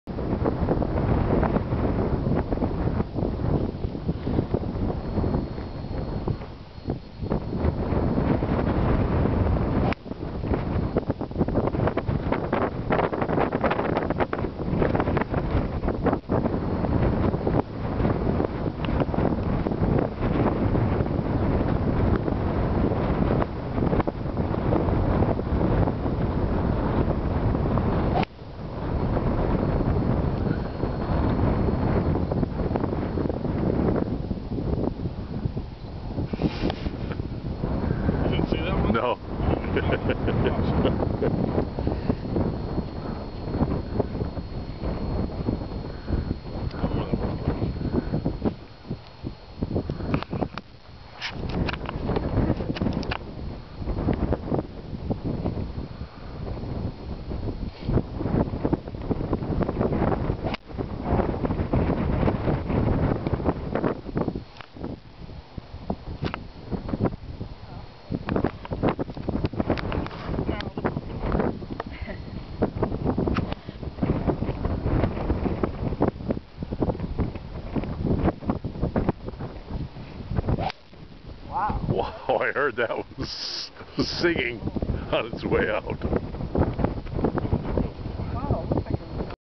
Gianni at Highland Links
Hitting balls into the Atlantic from the 6th fairway at Highland Links in Truro, 1/6/08.